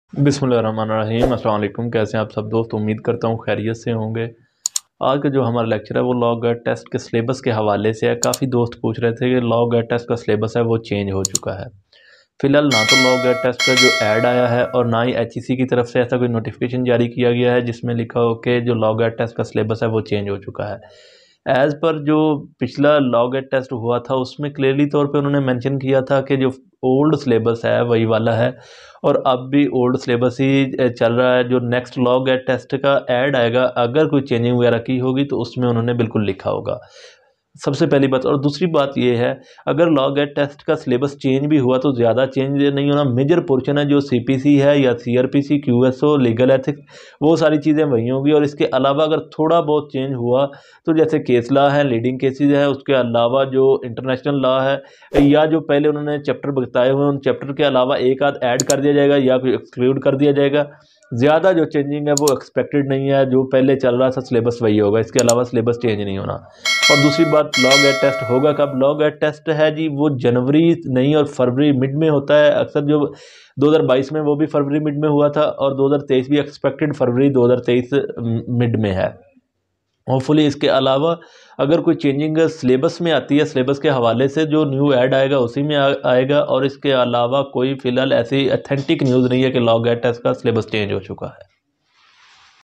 WHEN NEXT LAW GAT 2023 SYLLABUS IMPORTANT INFORMATION REGARDING TEST HEC ELIGIBILITY CRITERIA?
WHEN NEXT LAW GAT 2023 WILL BE CONDUCTED IMPORTANT INSTRUCTIONS REGARDUING SYLLABUS TEST PREPARATION ETC